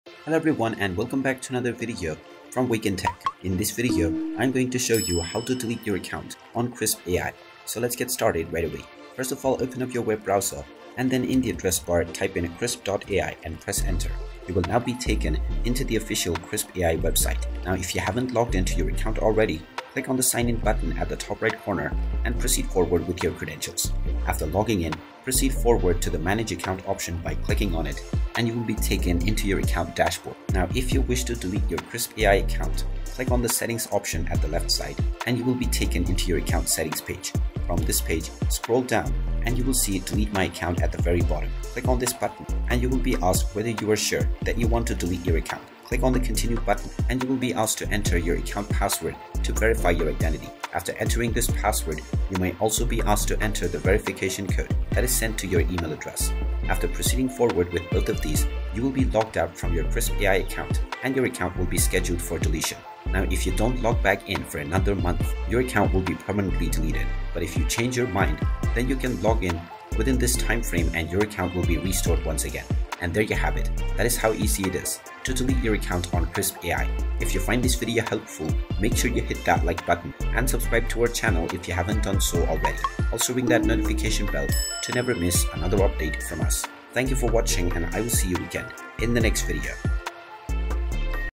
How to Delete Your Account on Krisp AI? Remove Your Account on Krisp AI on PC 2024
In this video, we'll guide you through the process of permanently deleting your Krisp AI account. Whether you’re looking to switch to a different service or just want to close your account, this step-by-step tutorial will help you do it quickly and easily.